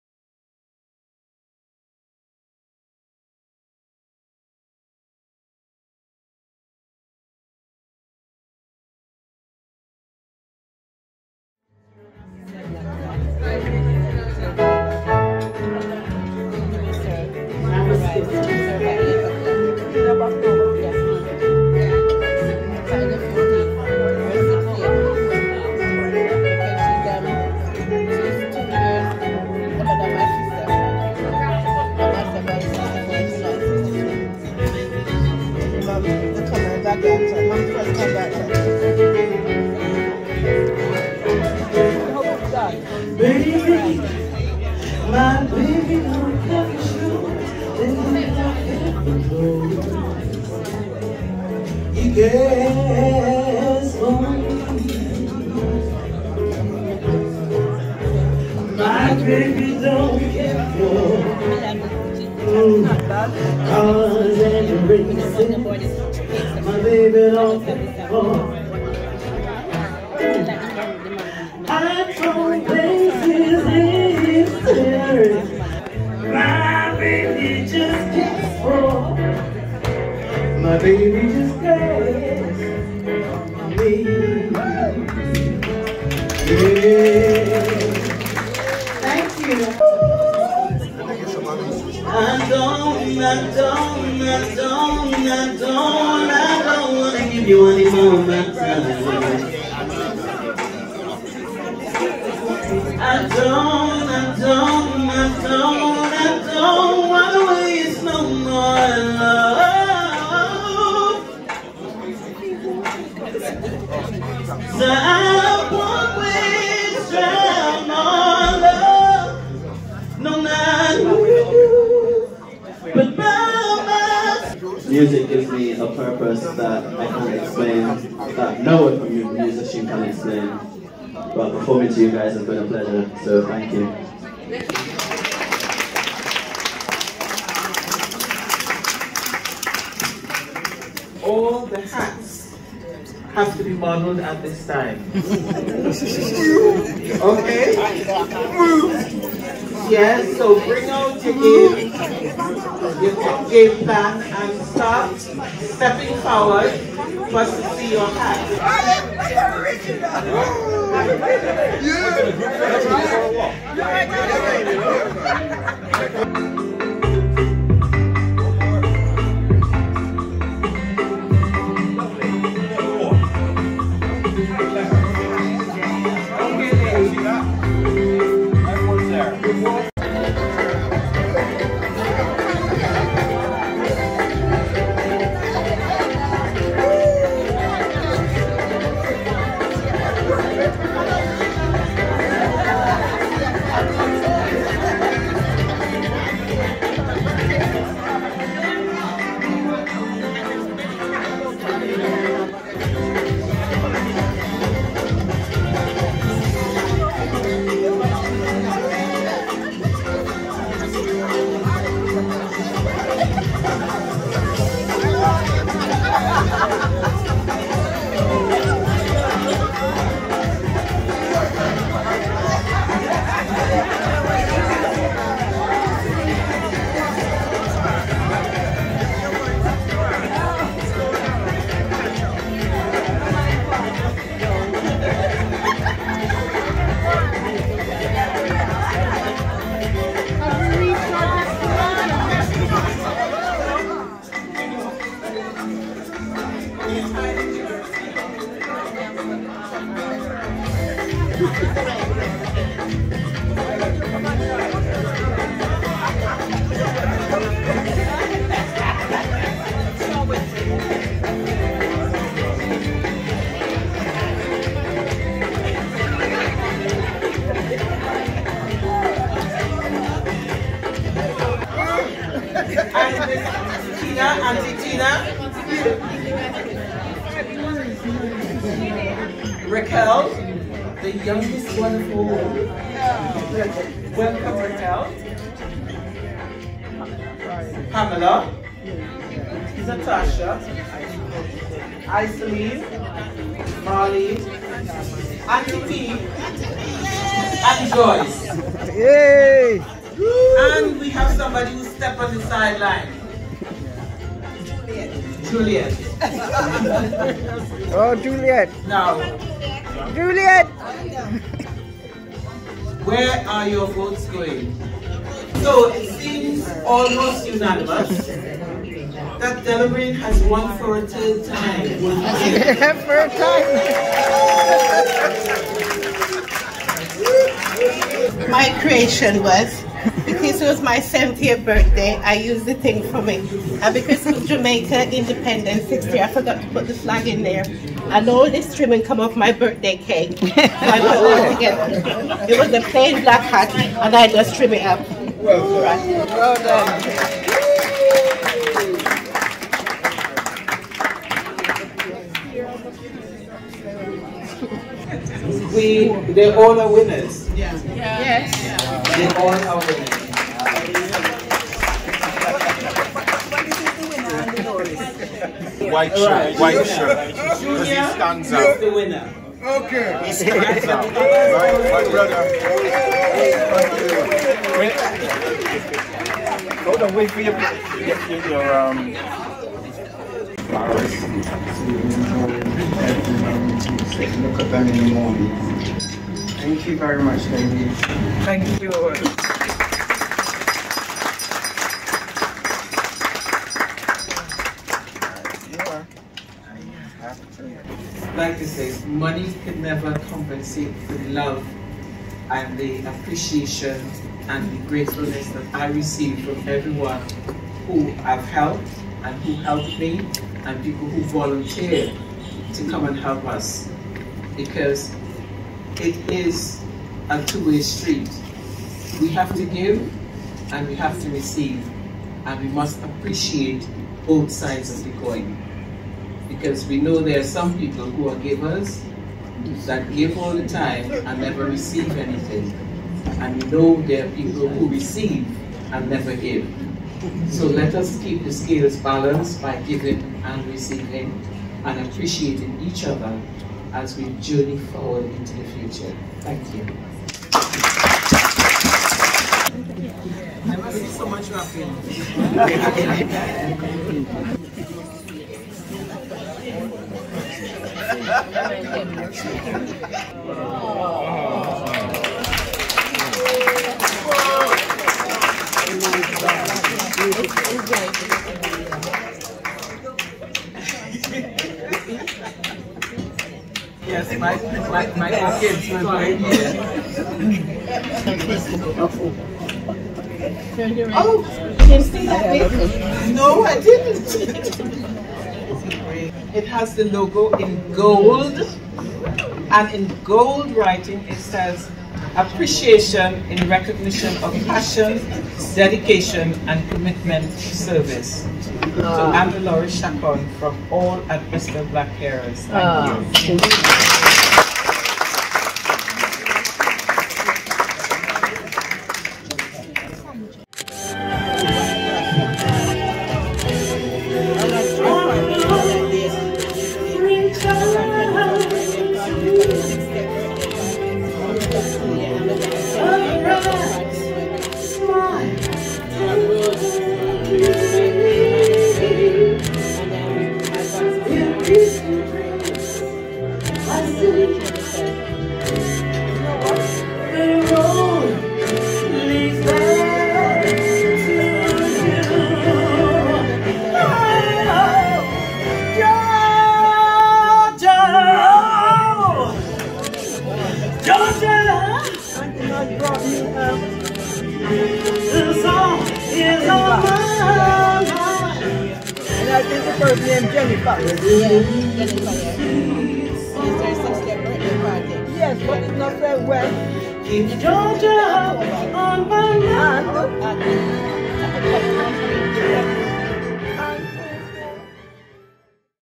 Bristol Black Carers Brims & Bows Hi Lights 2022
This short video was compiled as a quick ting from some random footage I took at the respected Bristol Black Carers annual 'Brims & Bows' event.
Bristol Black Carers was also celebrating its 25th anniversary. It was a lovely afternoon tea party full of fun, laughter, entertainment, facts & lovely sandwiches & little cakes.